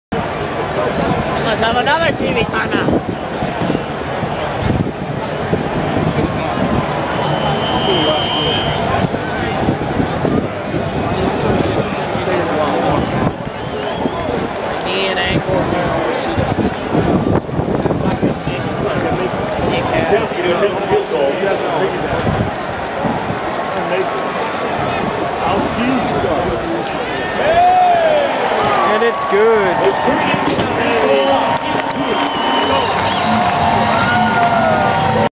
AFC divisional playoff gostkowski final points
Stephen Gostkowski kicks a field goal with less than a minute left in the patriots 41-28 win over the Texans